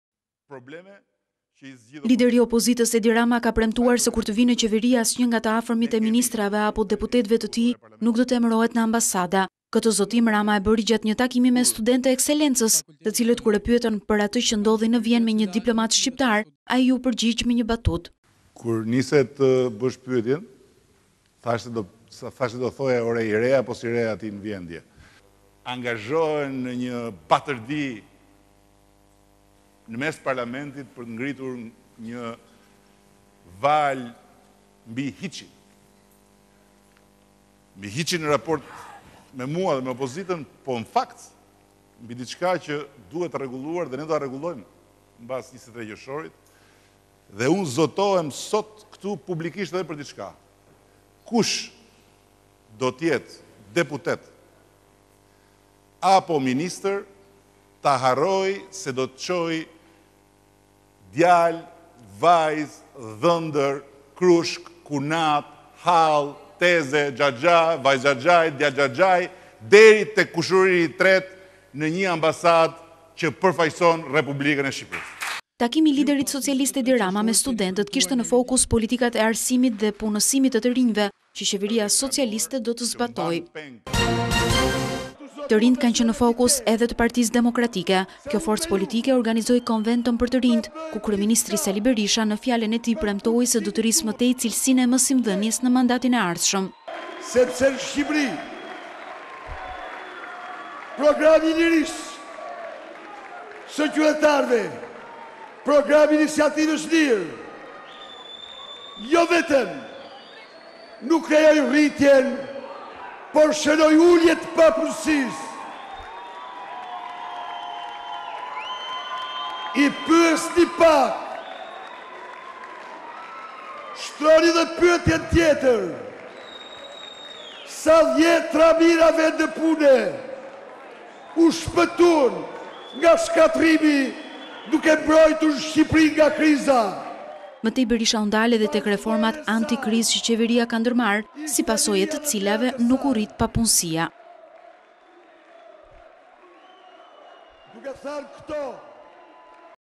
RAMA DHE BERISHA-TAKIME ME RININE FOKUSOHEN TE POLITIKAT MBESHTETESE-LAJM
7NEWS - kanal informativ Shqiptar